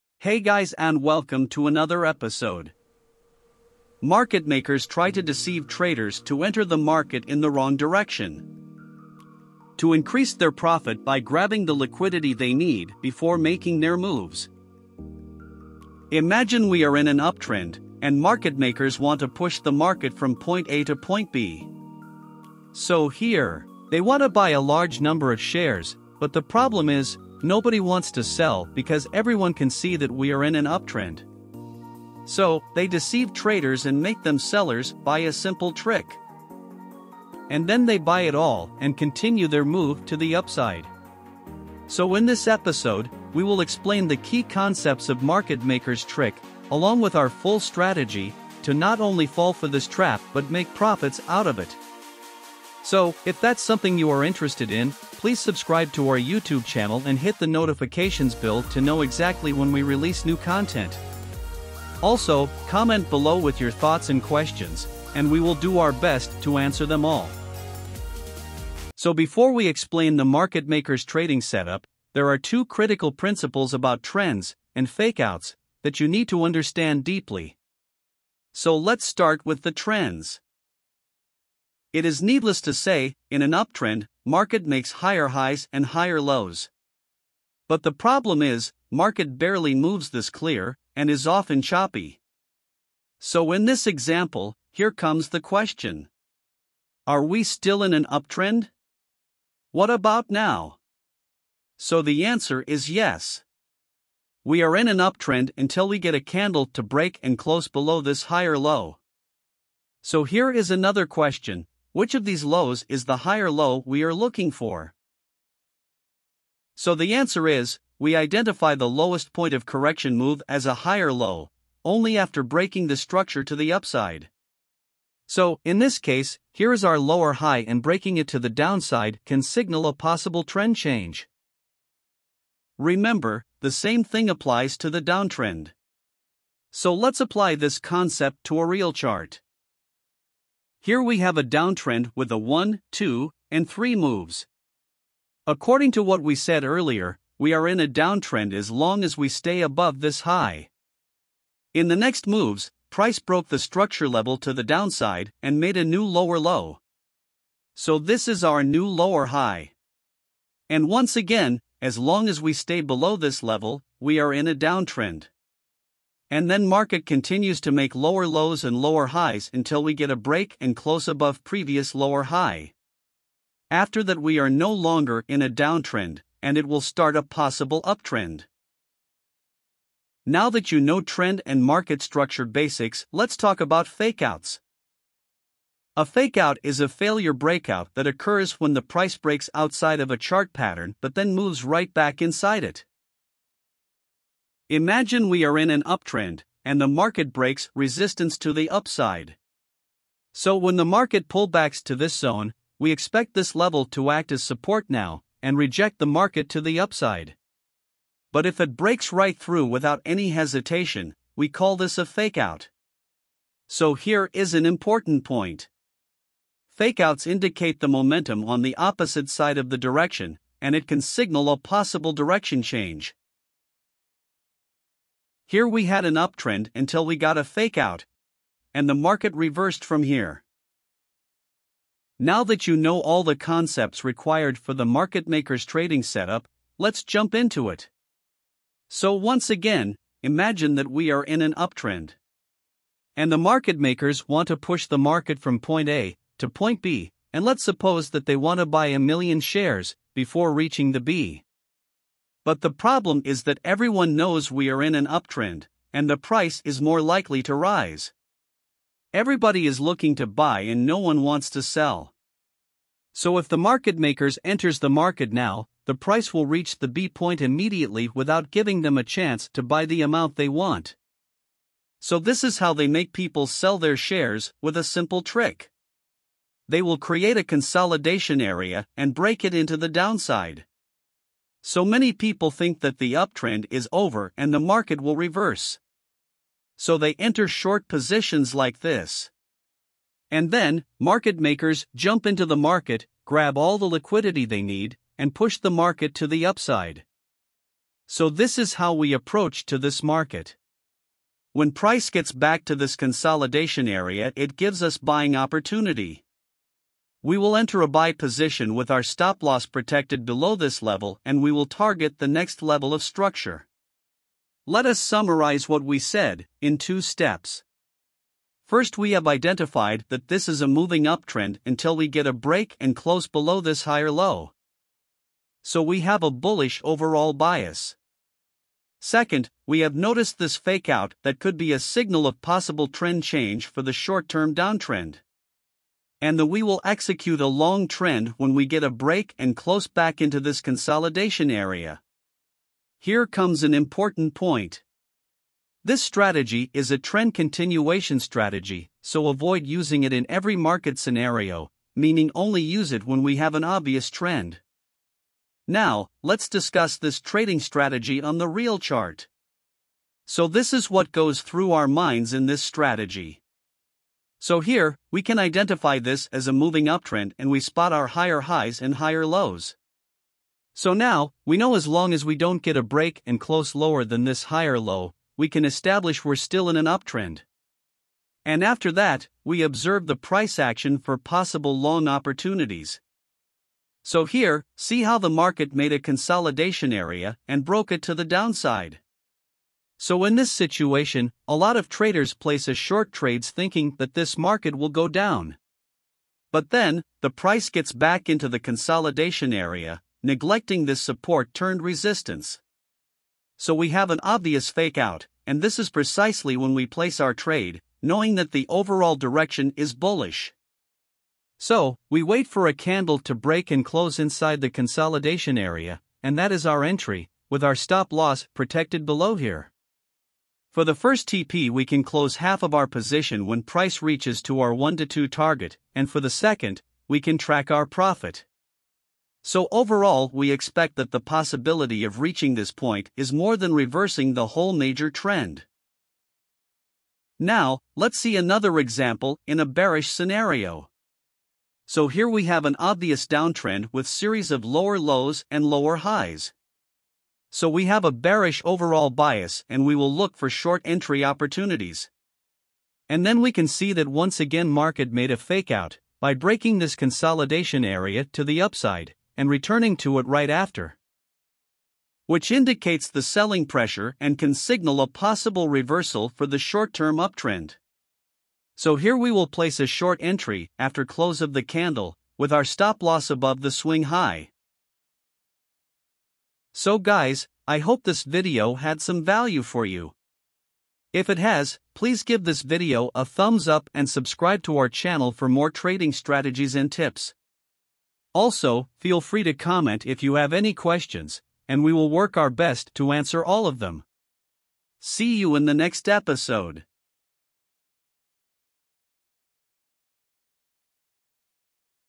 Market Makers Trading Strategy
Market makers try to deceive traders into entering the market in the wrong direction. to increase their profit by grabbing the liquidity they need before making their moves.
So in this episode, we will explain the key concepts of market makers' tricks along with our full strategy, to not only you won't fall for this trap and make profits out of it.